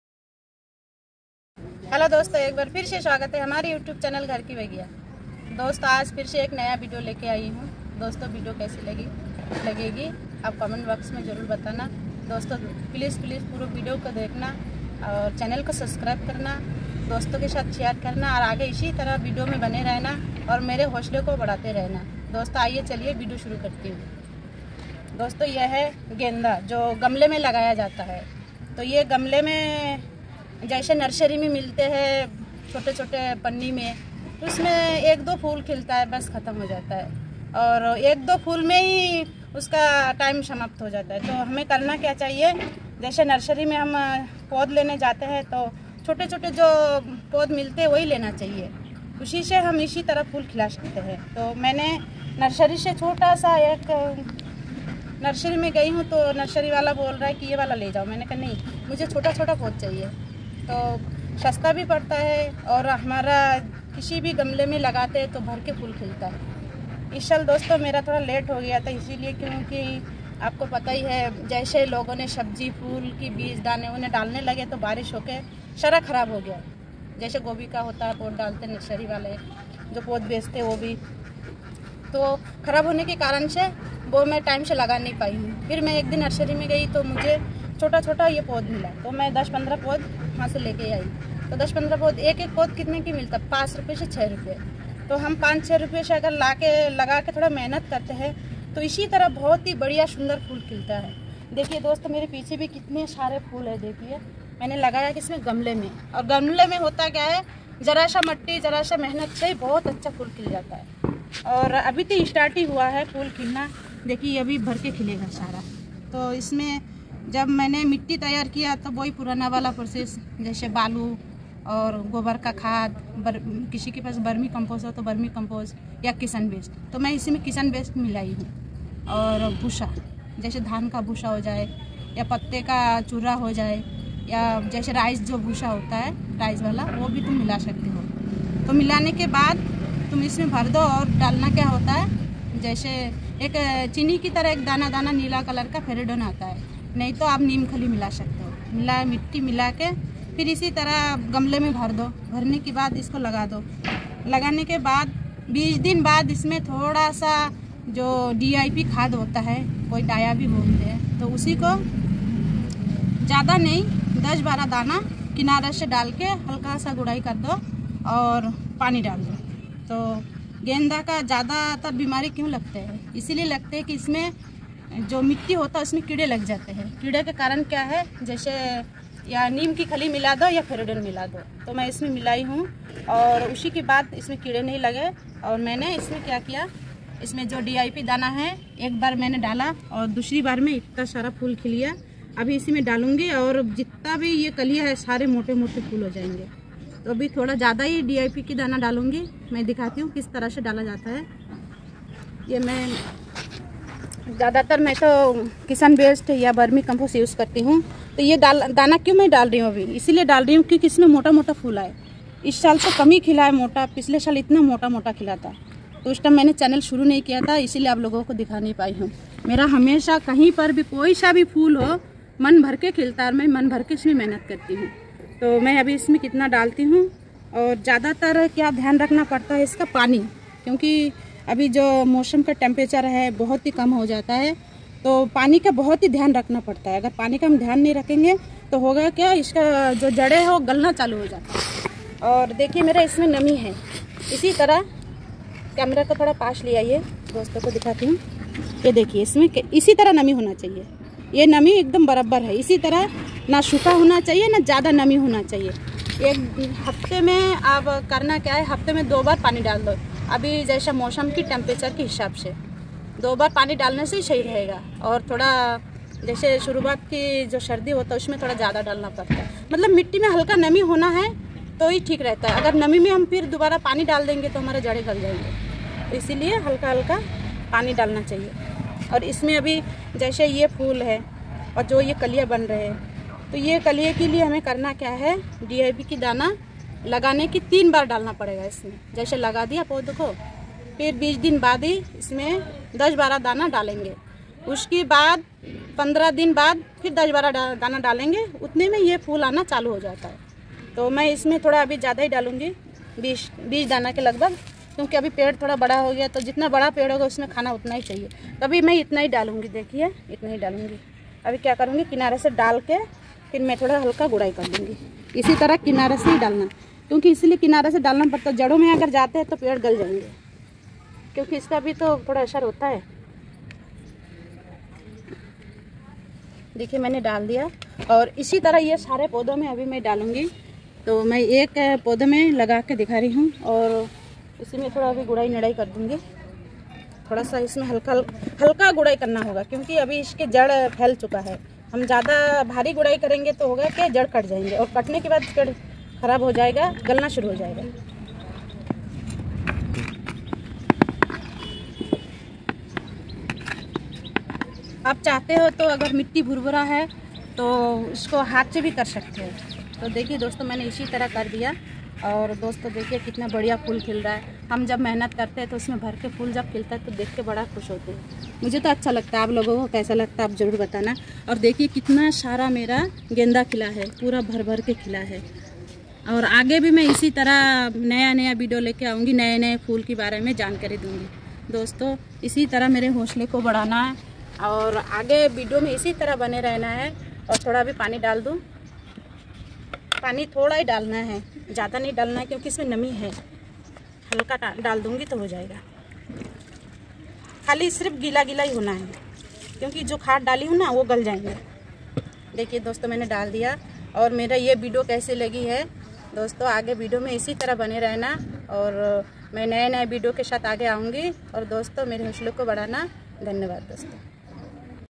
गैदे की कटिंग से खिल उठेंगे आपके बगीचे।। बस एक बार ये कर लीजिये।।@hamaribagiya22
एक बार फिर से स्वागत है आपका हमारे यूट्यूब चैनल घर की बगिया पर। इस वीडियो में हम आपको बताएंगे कि कैसे आप अपने लड्डू गैदे  के पौधे  की देखभाल कैसे
 करते हैं।

 धन्यवाद।